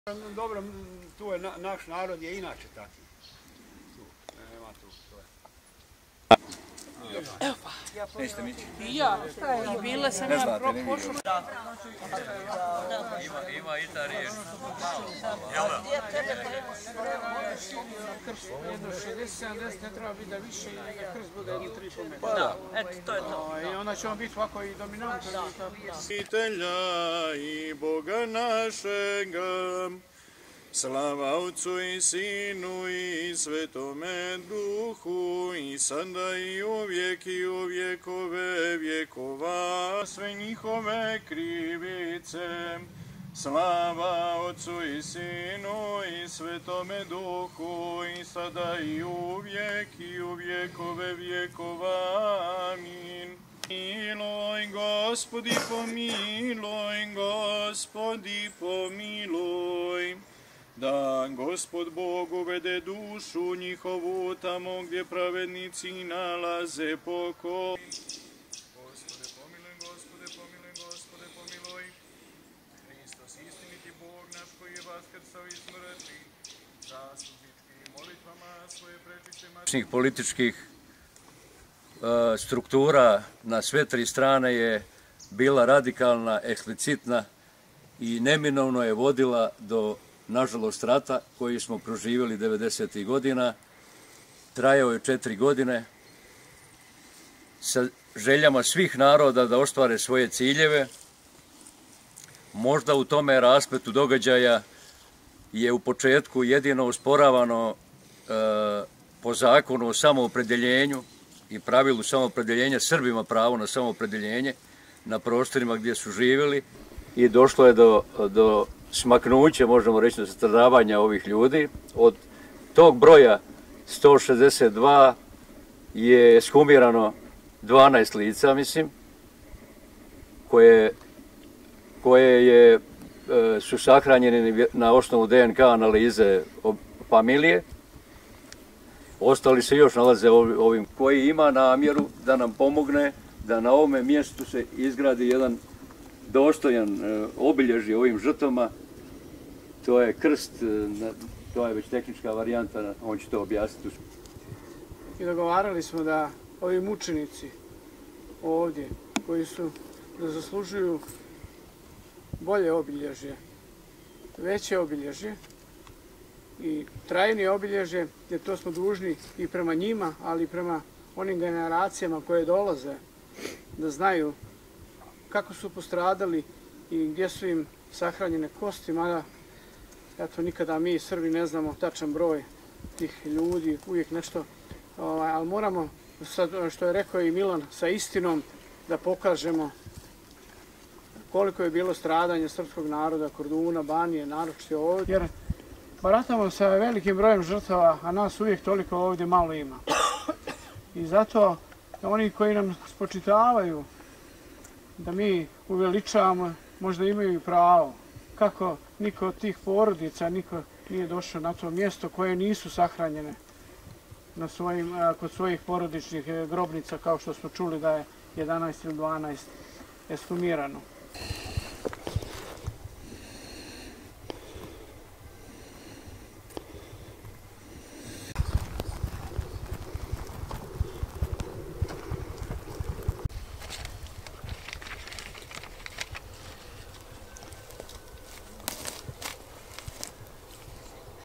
Parastos za srpske civilne žrtve stradale tijekom akcije Oluja na Banovini
Na groblju sv. Katarine paroh glinski jerej Goran Kalamanda održao je parastos za srpske civilne žrtve stradale tijekom VRO akcije Oluja na Banovini.